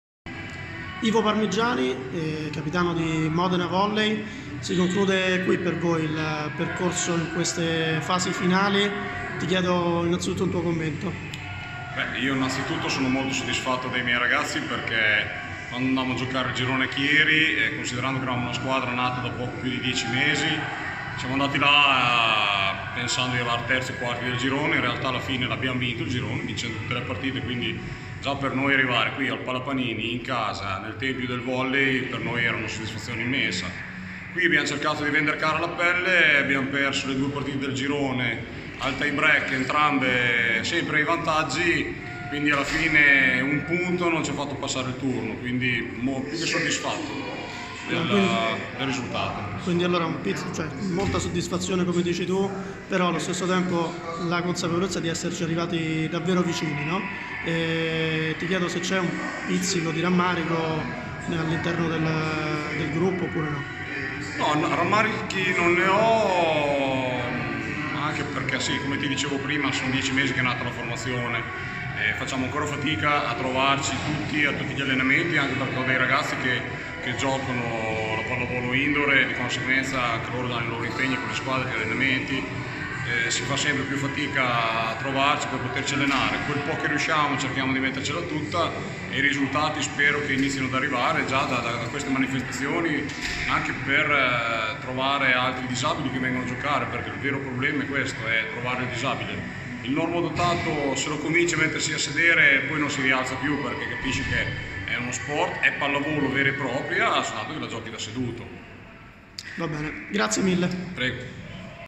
Finali SV tricolore: Ivo Parmiggiani commenta il sesto posto di Modena
Modena 2019: il capitano di Modena Volley Ivo Parmiggiani commenta le prestazioni della sua squadra al termine della finale 5°-6° posto persa contro Aversa.